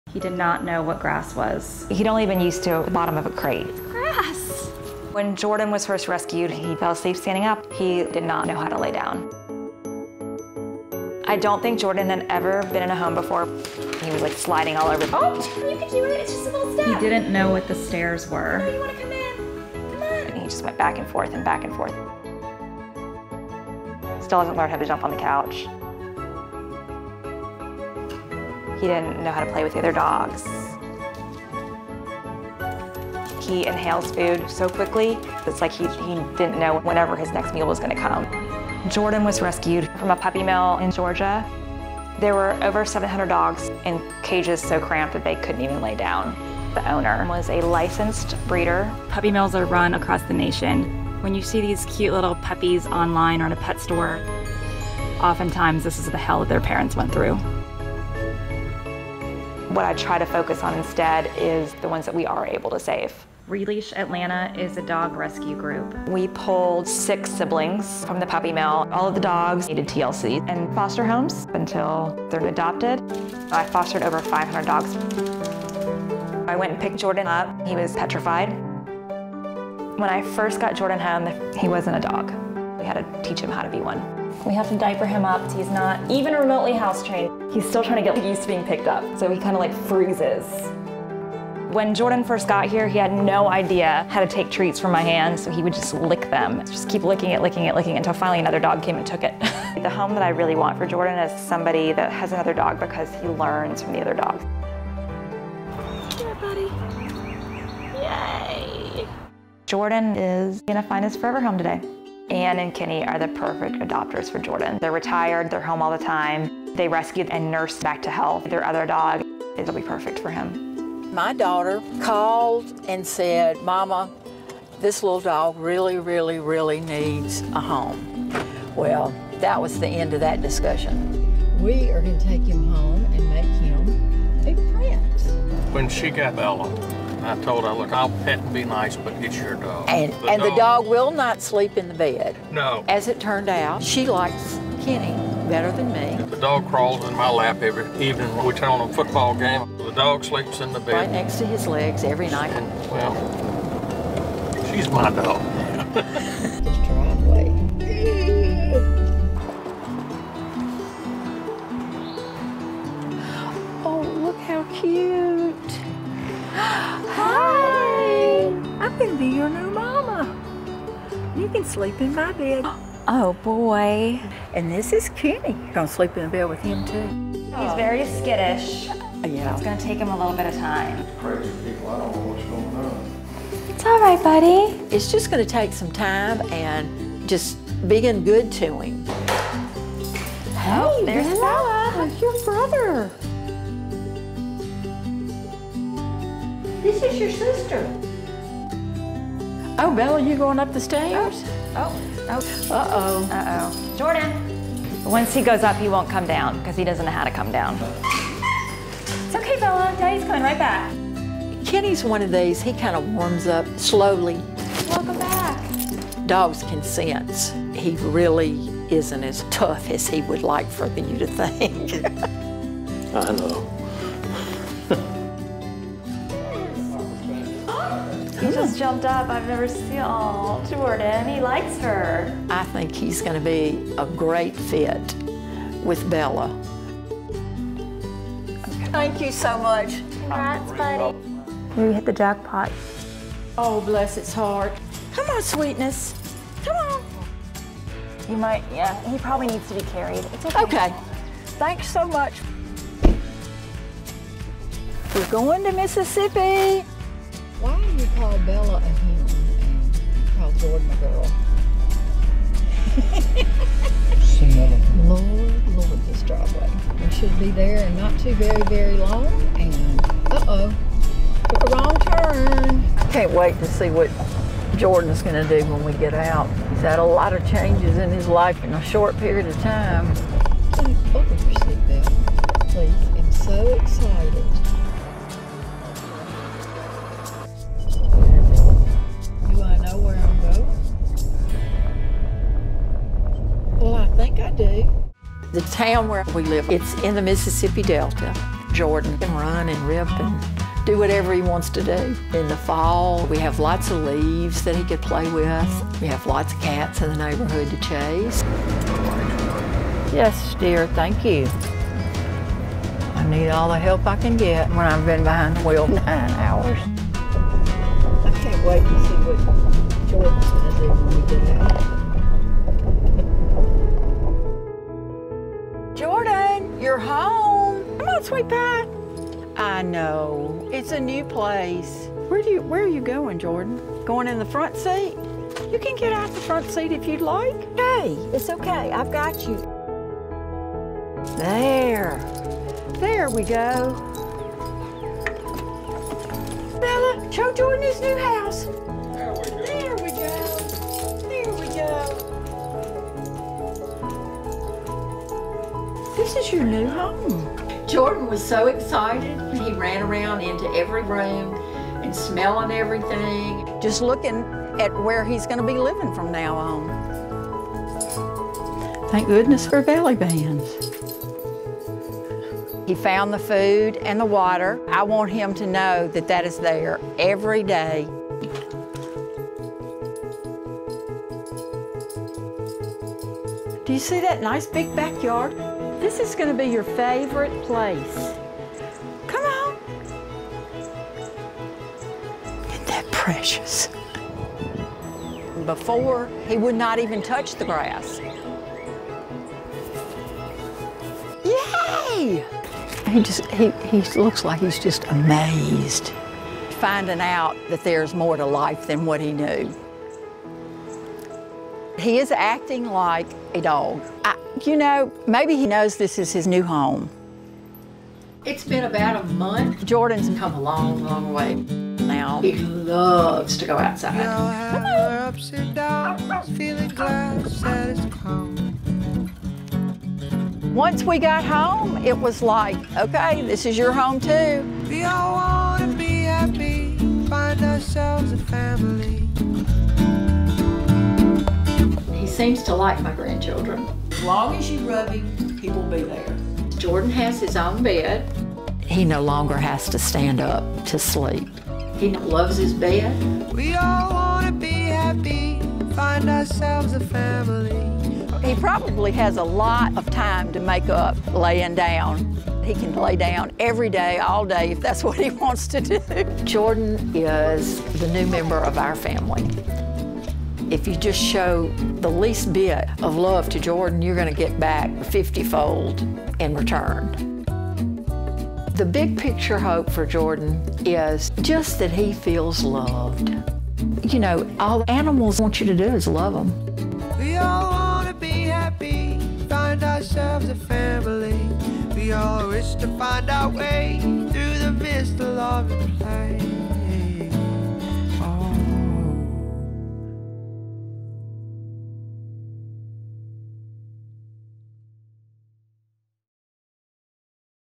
This Puppy Mill Dog Is Finally About To Get A Family | The Dodo Adoption Day
This dog was rescued from a puppy mill and didn't even know how to lie down. Now, he's finally getting adopted so he can live the life a dog deserves.

To help a dog like Jordan, you can support Releash Atlanta You can also check out Releash Atlanta on Facebook